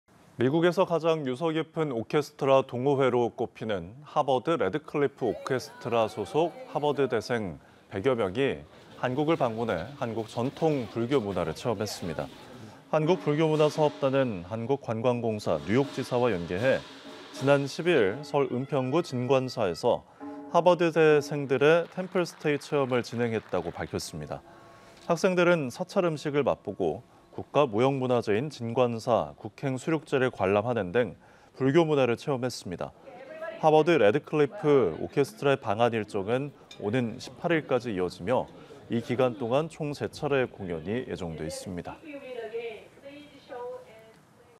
한국 찾은 하버드대 오케스트라단…진관사서 템플스테이 / 연합뉴스TV (YonhapnewsTV)
한국 찾은 하버드대 오케스트라단…진관사서 템플스테이

미국에서 가장 유서 깊은 오케스트라 동호회로 꼽히는 하버드-래드클리프 오케스트라 소속 하버드대생 100여명이 방한해 한국 전통 불교 문화를 체험했습니다.

한국불교문화사업단은 한국관광공사 뉴욕지사와 연계해 지난 12일 서울 은평구 진관사에서 하버드대생들의 템플스테이 체험을 진행했다고 밝혔습니다.

학생들은 사찰음식을 맛보고, 국가무형문화재인 진관사 국행수륙재를 관람하는 등 불교 문화를 체험했습니다.

하버드-래드클리프 오케스트라의 방한 일정은 오는 18일까지 이어지며, 이 기간 총 세 차례의 공연이 예정돼 있습니다.